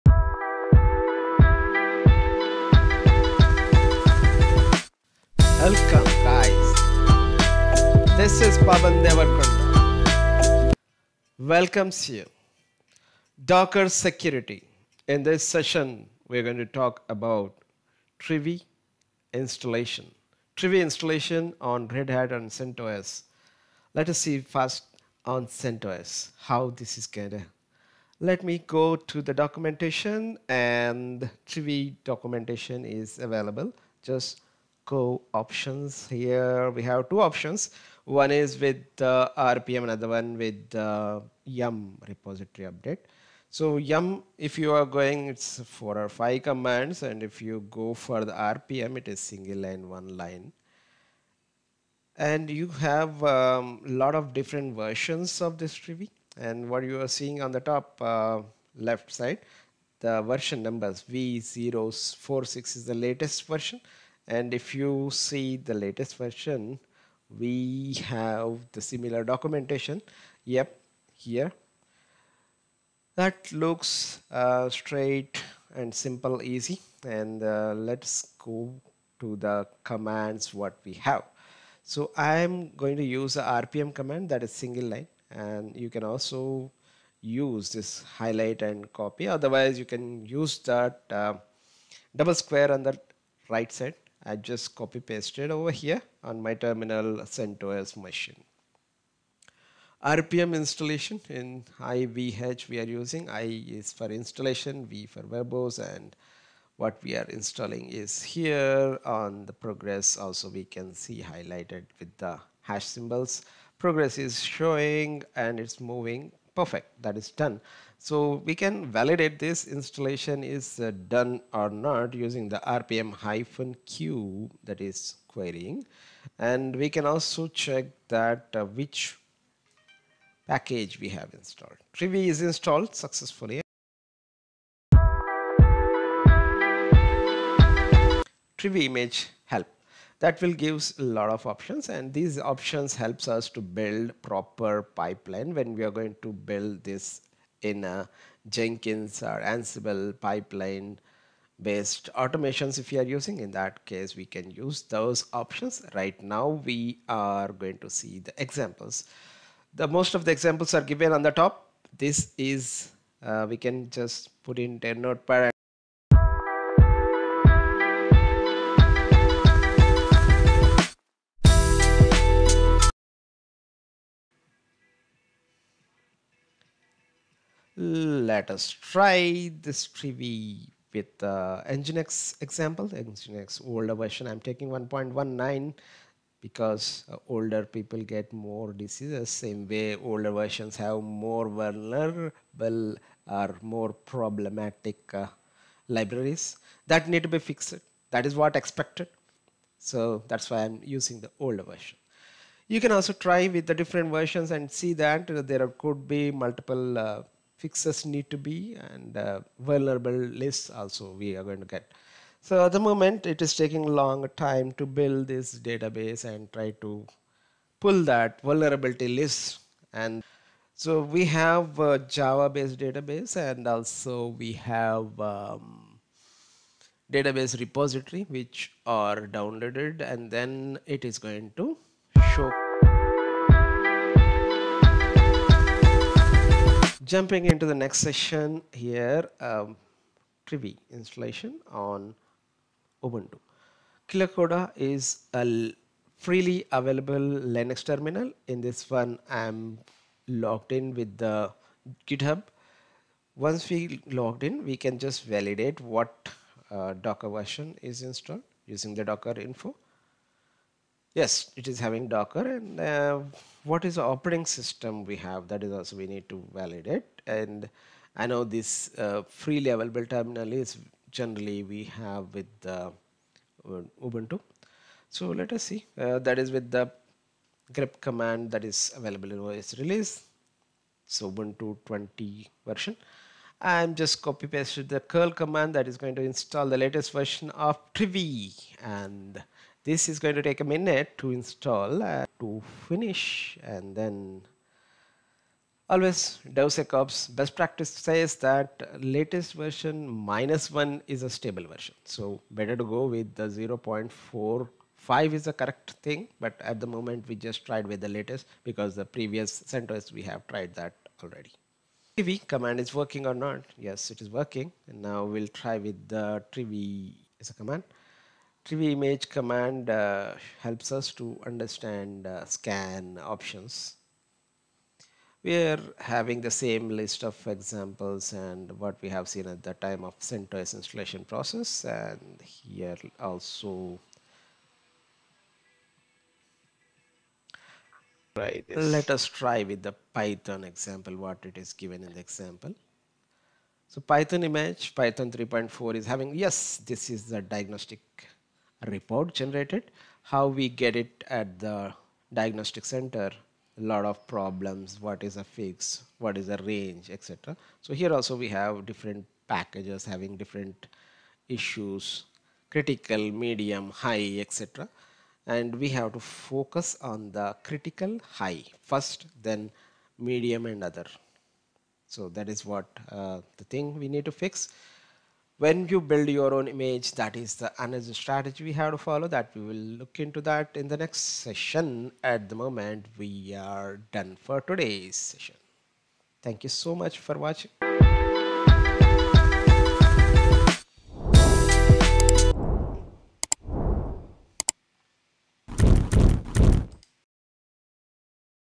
Aqua Trivy image scanner install on CentOS and Ubuntu | Docker Security | DevSecOps tool
CentOS Ubuntu Securityscan securitytutorial my experiments with Trivy container security scanning free tutorial
Basic Detailes of understanding to learn how to identify security vulnerabilities on Docker container image.